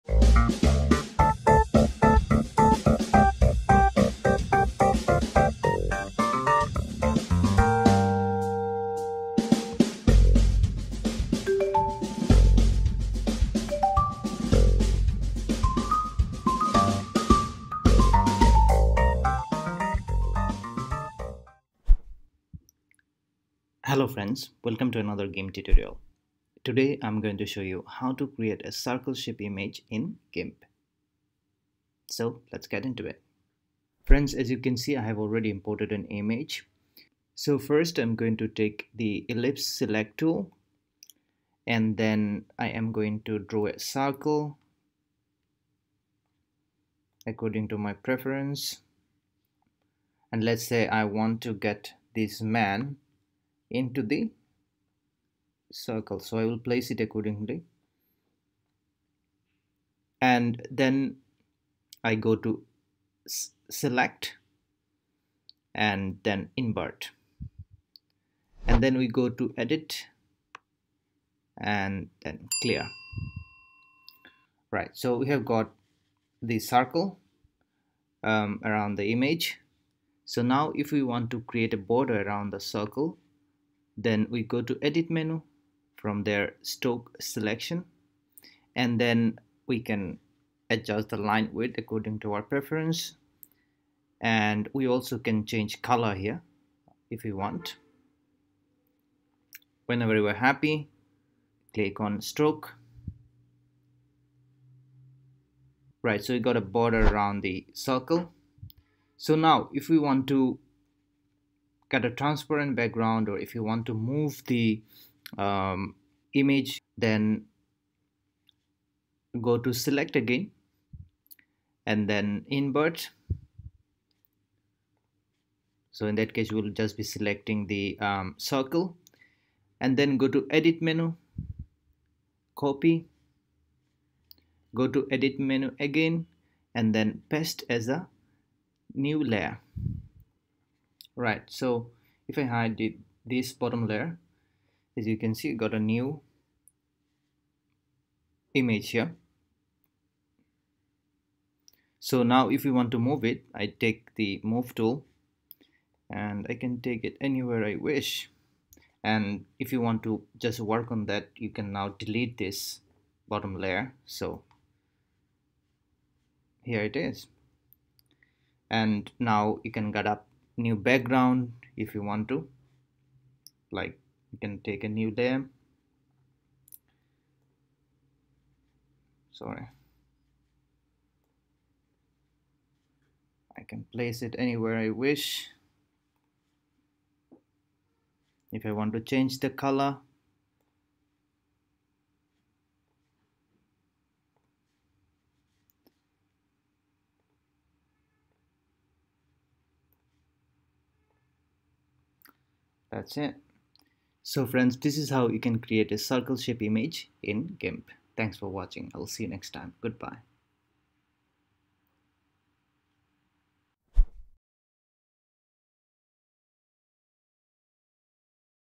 How to Create Circle Shape Image in Gimp | Tutorial-30
Hello friends,

Welcome to another gimp tutorial. In this video, I'm going to show you how to create a circle shape image in Gimp.

Thanks
LI